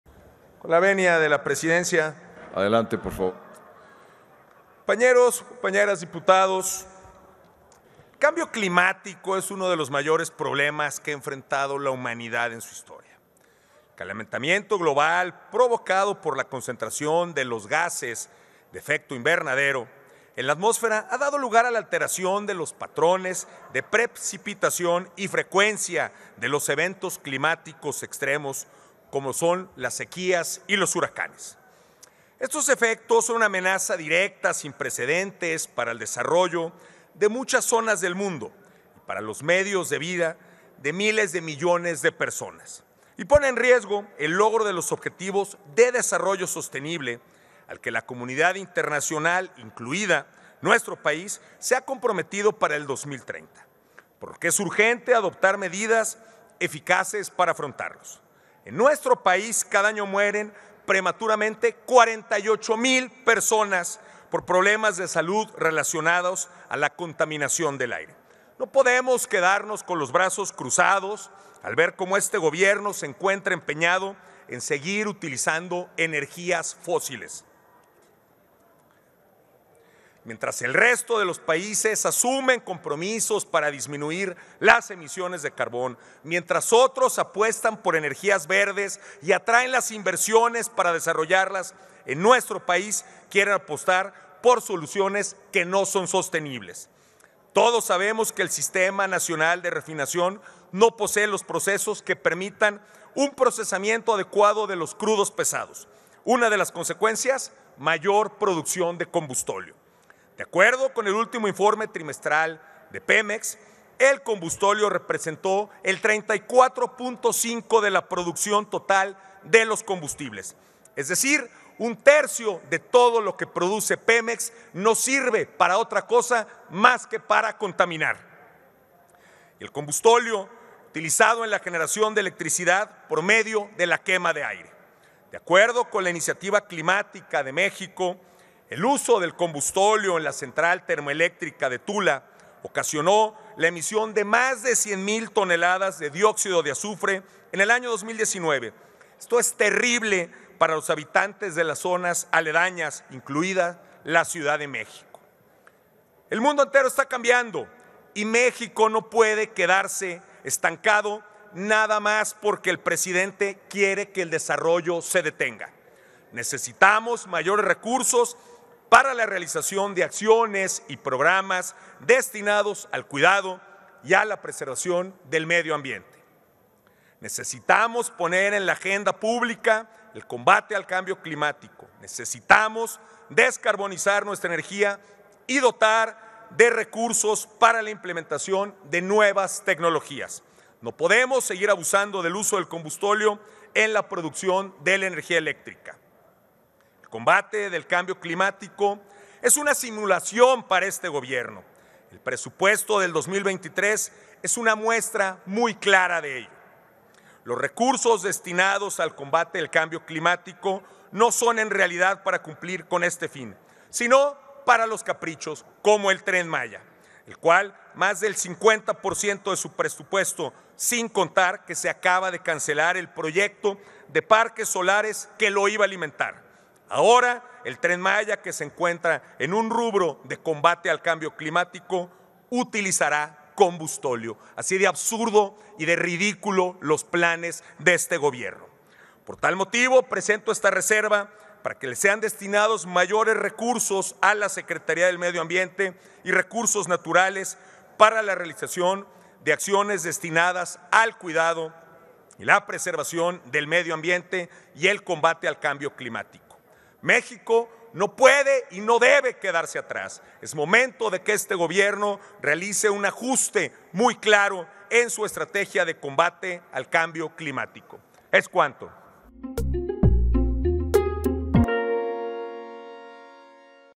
Dip. José Mauro Garza Marín (MC) / Reserva al dictamen de la Ley Federal de Derechos.
El diputado José Mauro Garza Marín (MC) presenta reserva al dictamen que reforma, adiciona y deroga diversas disposiciones de la Ley Federal de Derechos.